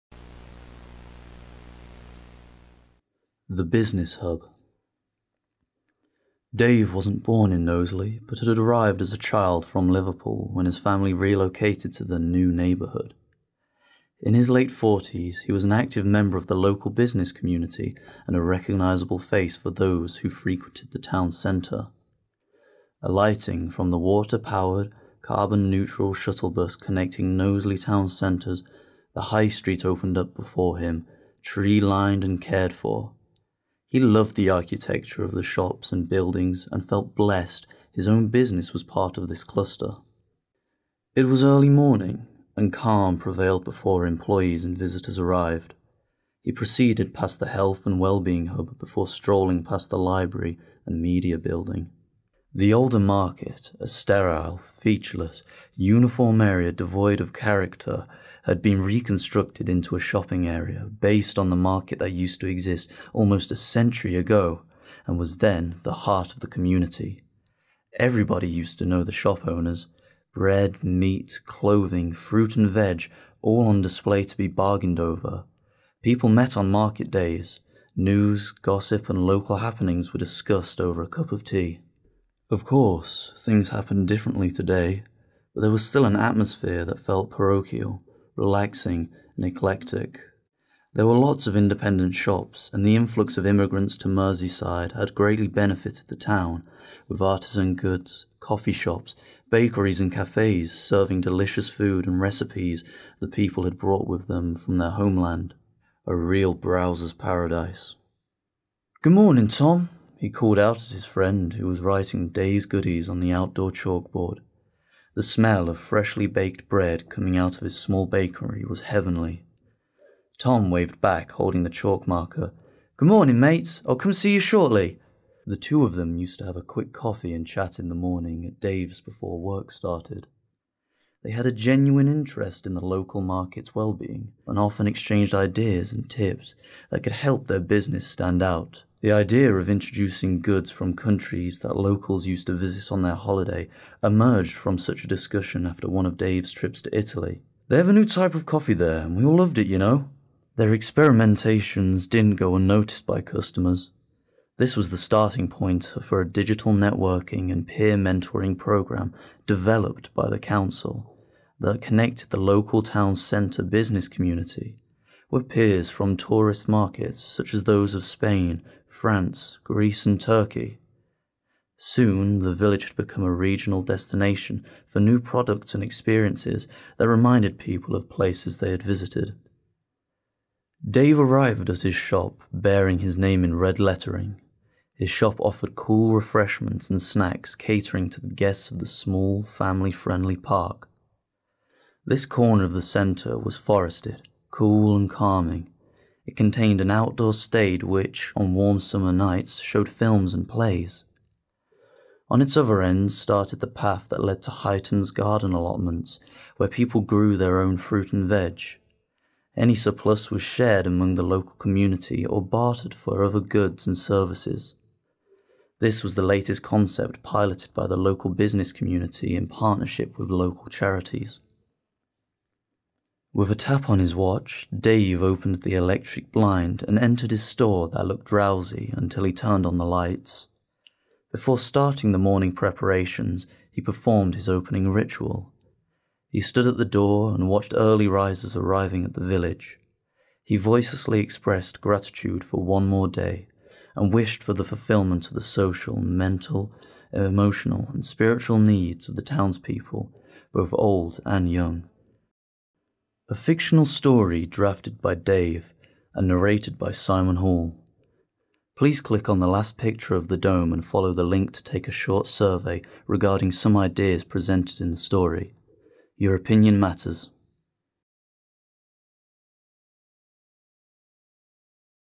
The Business Hub
Dave (pseudonym) is a keen traveller who gets inspired by interacting with people from around the world. In his story, he arrives downtown early in the morning to open his shop. He is a dedicated local shop owner with a restless spirit and a desire to progress. His entrepreneurial spirit inspires the development of a business-to-business network with a flourishing effect on the local market.

From October 2021 to April 2022, LJMU PhD candidate Paschalis Chatzitryfonos delivered creative writing workshops in Knowsley, supporting community members to envision the future of the local town centre. "Future Huyton" presents their creative outcomes.